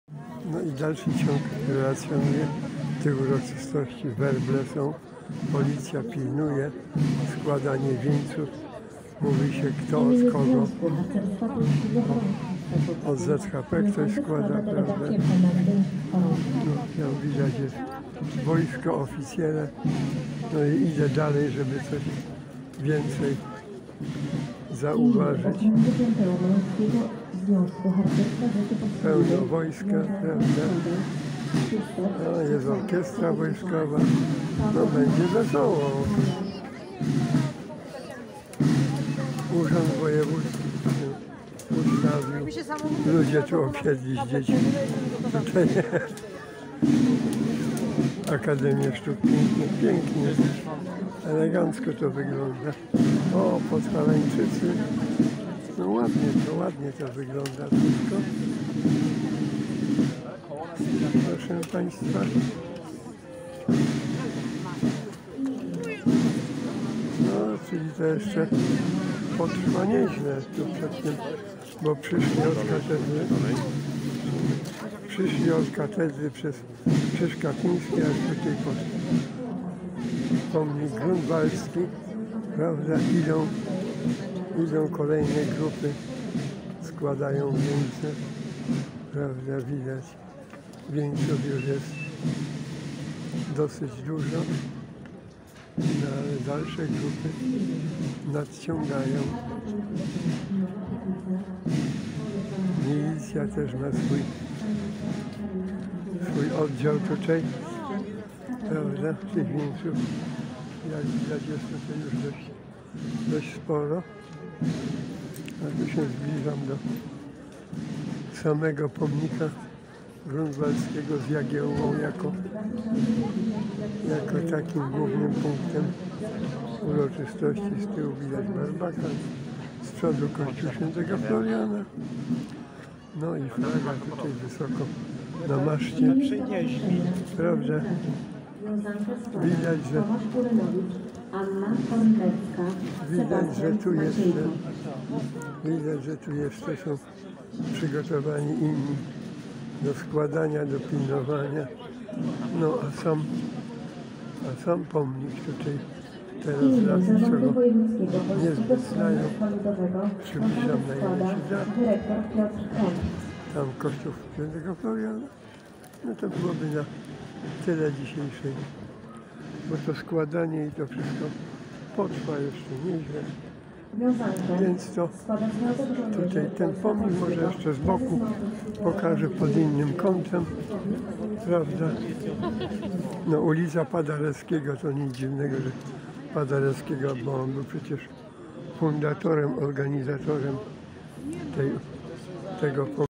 Cd UROCZYSTOŚCI 11 XI 21 przed POMNIKIEM Grunwaldzkim w Krakowie , Składanie Wieńców przez Oficjeli
Jesli chcesz drogi widzu mozesz wplacic dowolna kwote na rozwoj mojego kanalu tutaj procesja idzie ul. kanoniczną przez plac Marii Magdaleny k.pomnika Piotra Skargi/który niektórzy chcą usunąć/pod Okno Papieskie śpiewając przypisane pieśni.Przed Kurią odmawia się różaniec i koronkę.amen XXXXXXXXXXXXXXXXXXXXXXXXXXXXXXXXXXX
XXXXXXXXXXXXXXXXXXXXXXXXXXXXXXXXXXXX